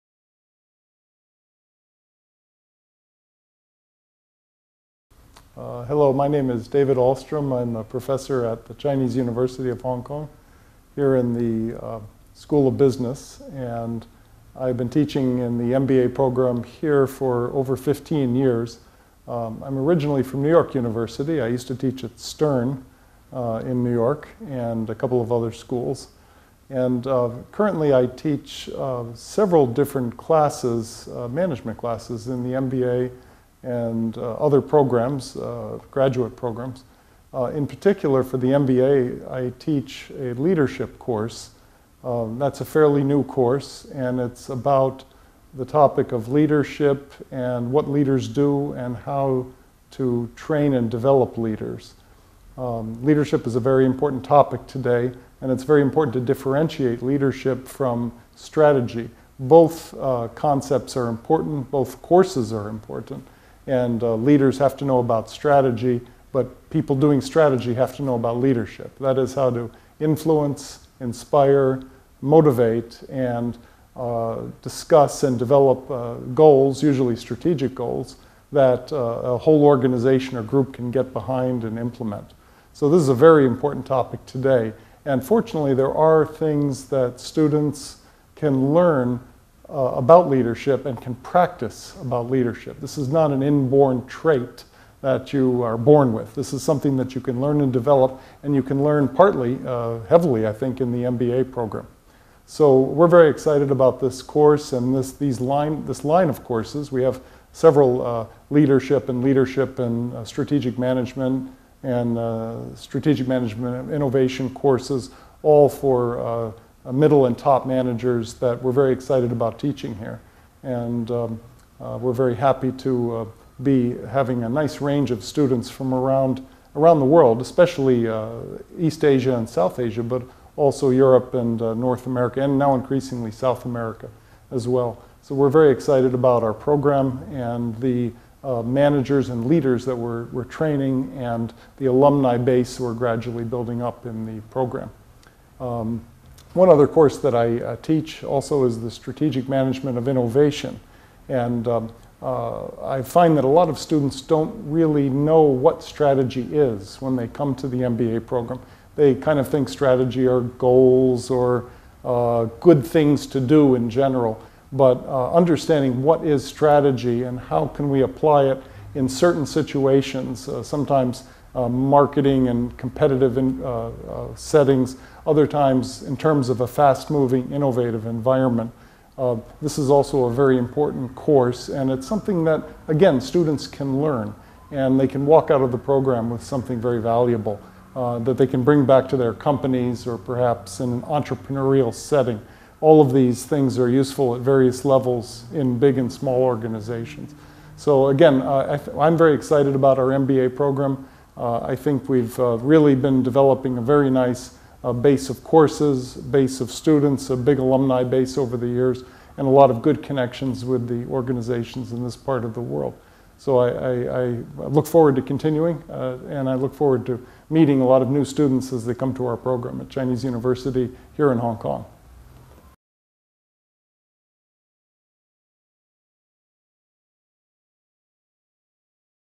Professor David Ahlstrom shares his idea on leadership and management teaching
Professor Ahlstrom has taught in Stern Business School, NYU and a couple of other schools. He has been teaching in CUHK MBA for more than 15 years to now. He now teaches several management classes including strategic management in MBA and other graduate programs.